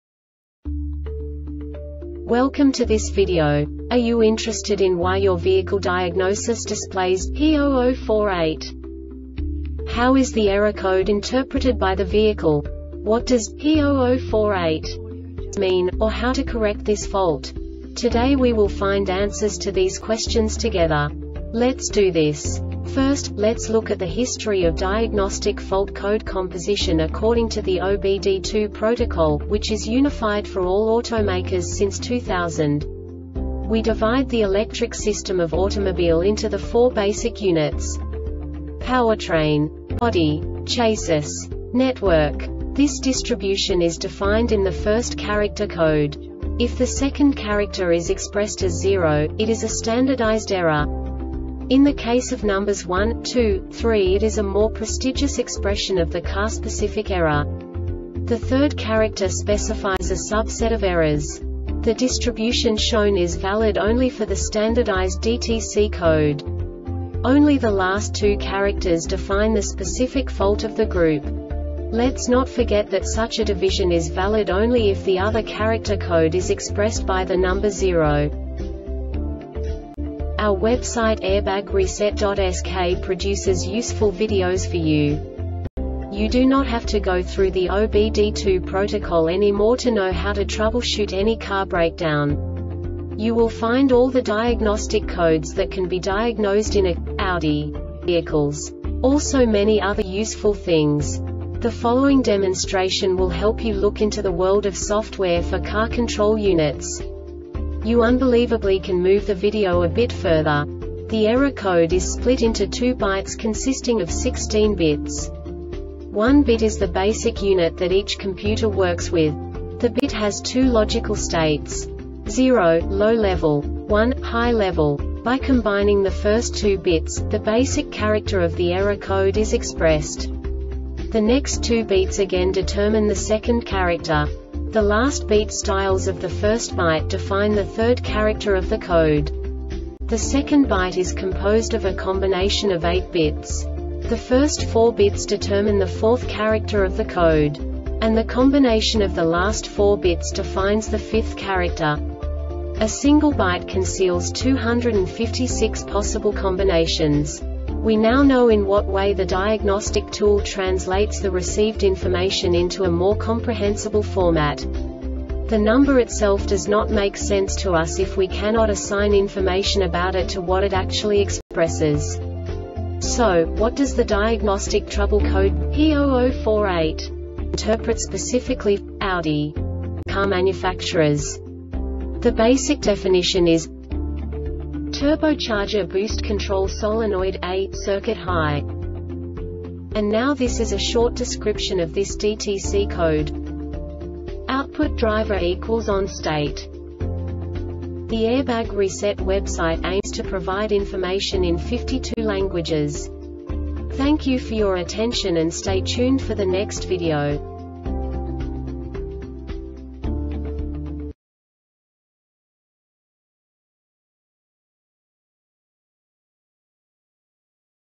DTC Audi P0048 Short Explanation
Contents:
0:21 Basic DTC analysis according to OBD2 protocol standard.
1:48 Insight into programming
2:58 A diagnostically understandable description of the Diagnostic Trouble Code
3:05 Basic error definition

Make:
Audi
Code:
P0048
Definition:
Turbocharger Boost Control Solenoid "A" Circuit High
Description:
Output driver-  equal  ON state